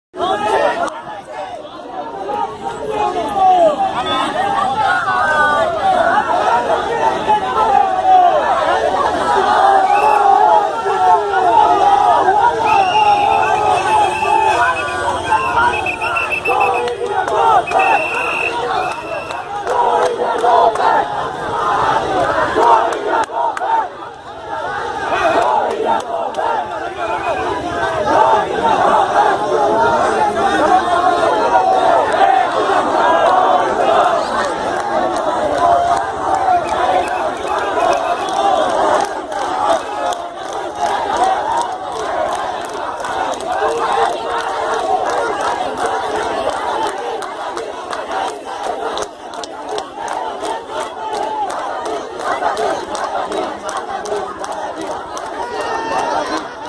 'Go India Go Back' : Anti-India Slogans at JNU
'Go India Go Back' by some Anti-Nationals students of JNU